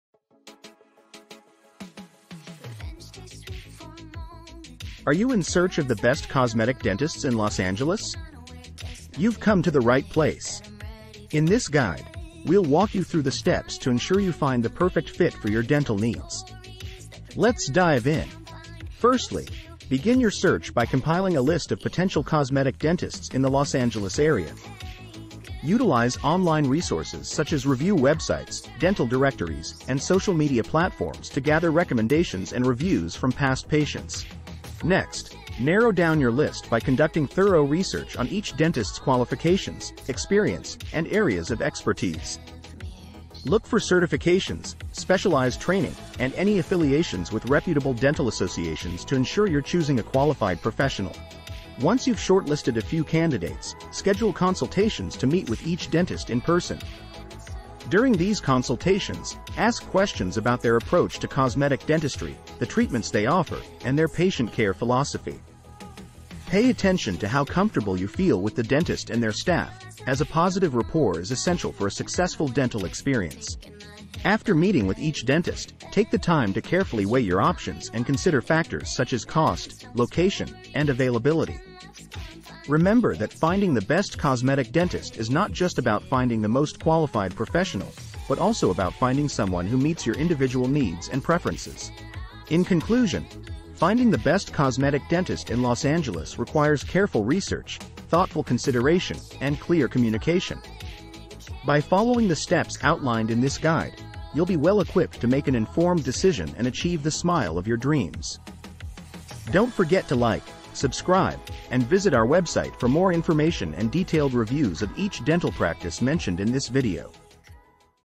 A Comprehensive Guide to Discover the Best Cosmetic Dentists in Los Angeles
Join us in this enlightening video guide as we unveil the secrets to finding the top cosmetic dentists in Los Angeles. From expert tips on researching to essential considerations, this video will equip you with the knowledge you need to make the best choice for your dental care needs.

Chapters:
00:00:00 - Introduction
00:00:18 - Compiling a List of Potential Cosmetic Dentists
00:00:35 - Conducting Thorough Research on Qualifications and Experience
00:00:52 - Scheduling Consultations and Asking Questions
00:01:14 - Weighing Options and Considering Factors
00:01:32 - Conclusion